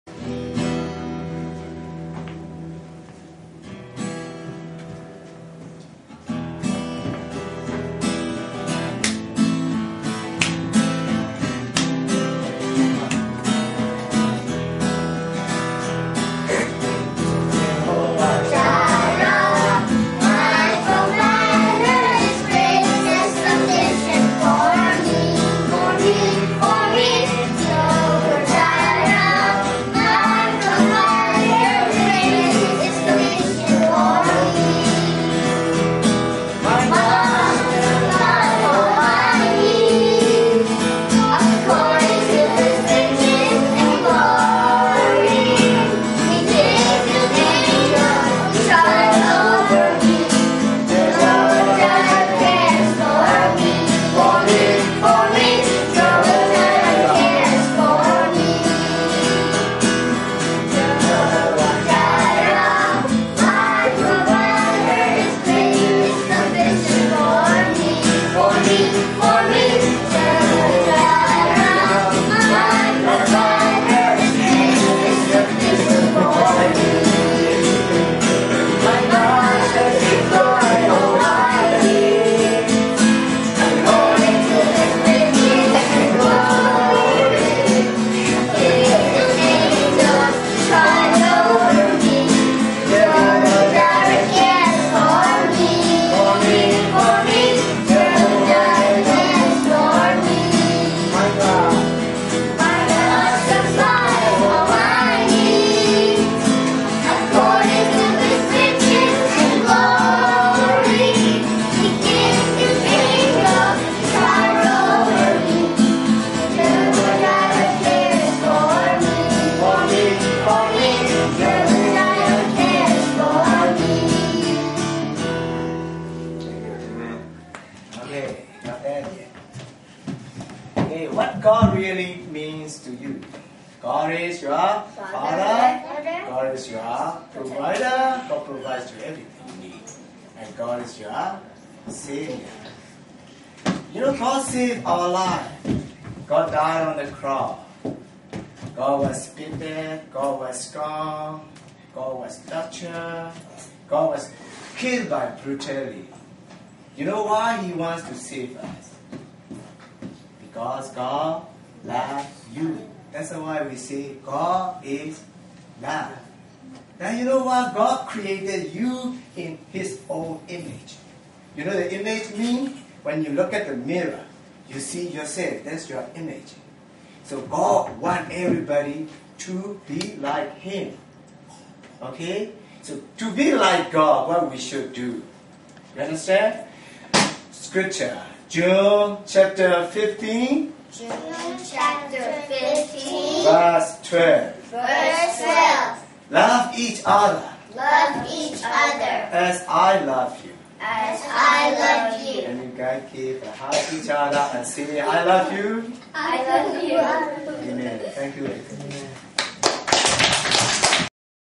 RCF Ottawa Sunday Worship (2008-08-03)
Revival Christian Fellowship (Ottawa) Sunday Worship (August 3, 2008)

John 15:12
Love each other as I have loved you.